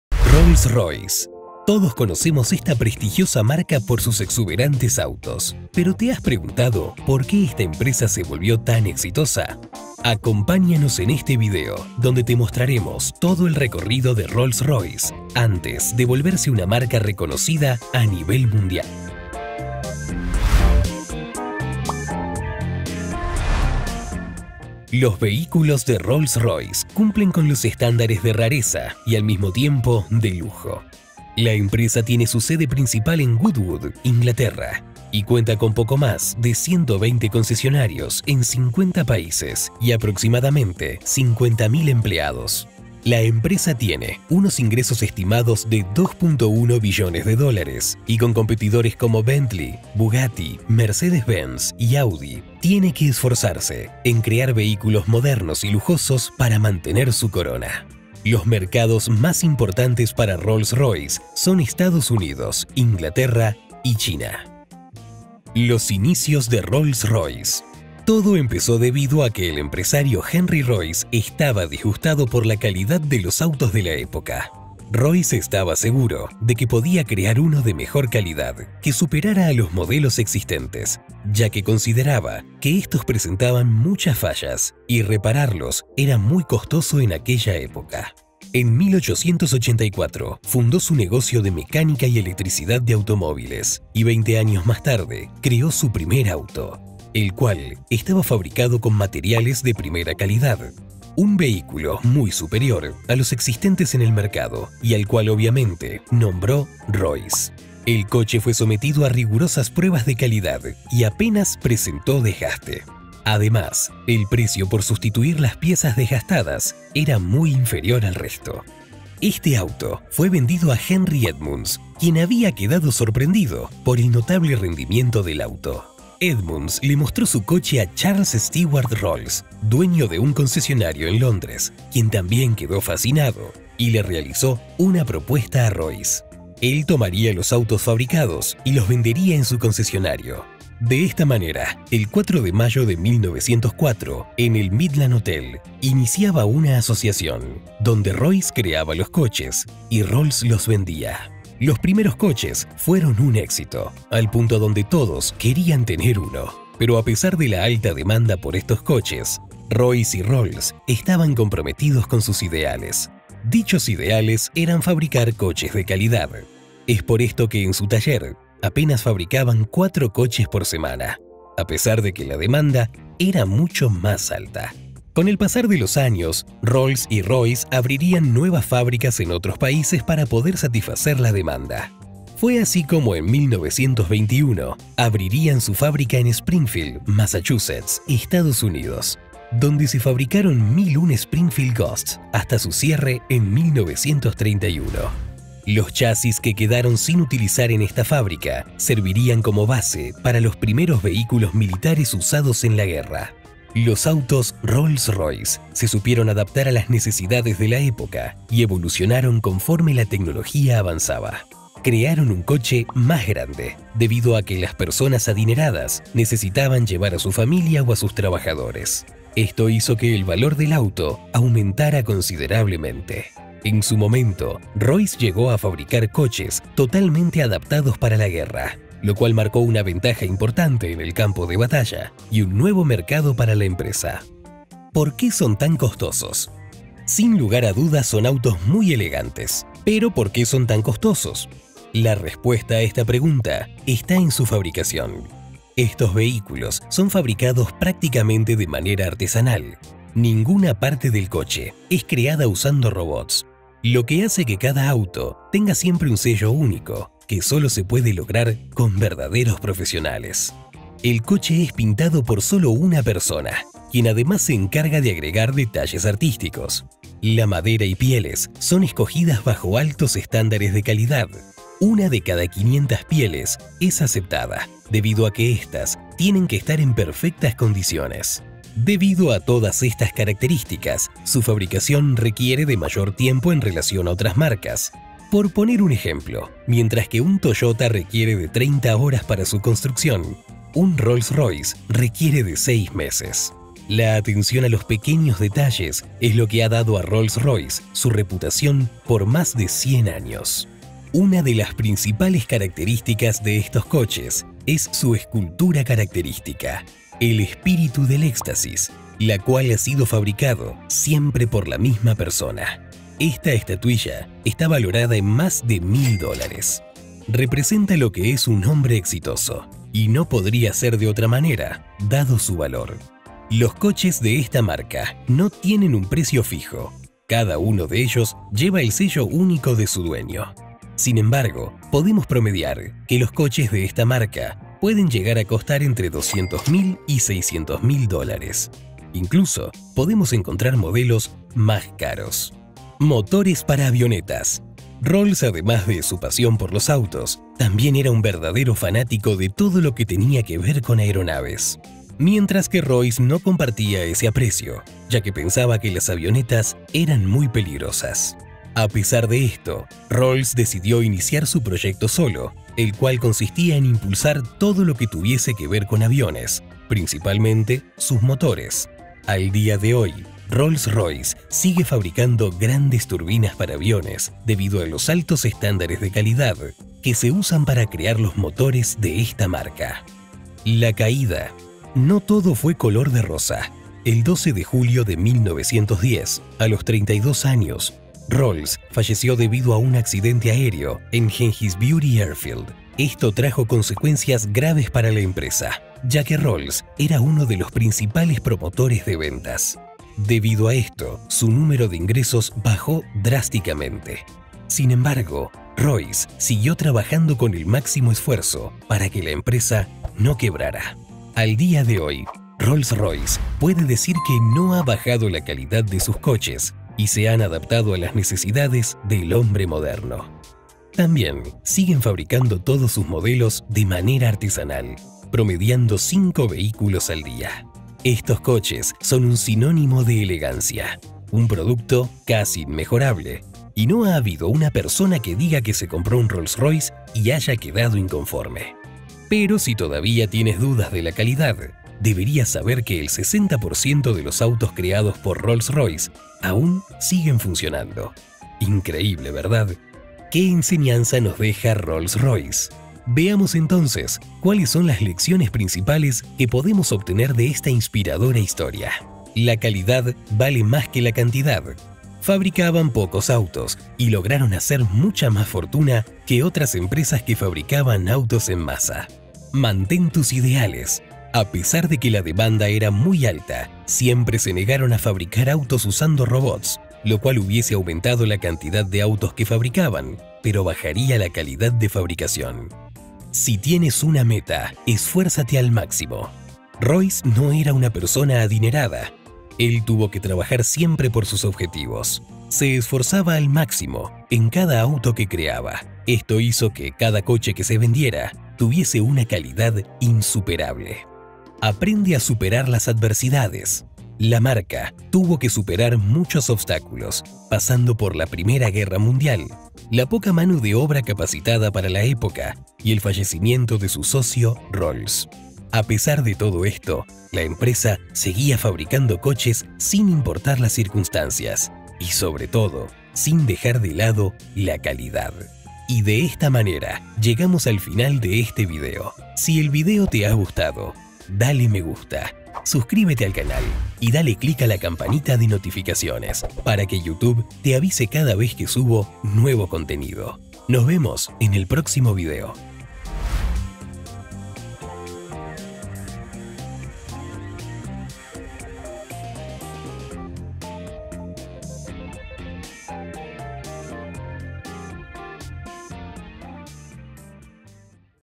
🚗 Como se Convirtió en el Rey de los Autos de Lujo 👑 | Caso Rolls Royce
Rolls Royce revolucionó la industria de los autos de lujo y a día de hoy es una de las empresas más reconocidas a nivel mundial, sus autos son sinónimo de lujo.

En este video veremos la historia de Rolls Royce, como llegó a ser lo que es, los obstáculos que tuvo que superar y si te quedas hasta el final verás que depara el futuro para esta inspiradora empresa automotriz.

LECCIONES:
1. La calidad ante todo.
2. Mantén tus ideales.
3. Esfuérzate al máximo para alcanzar tu meta.
4. Aprende a superar las adversidades.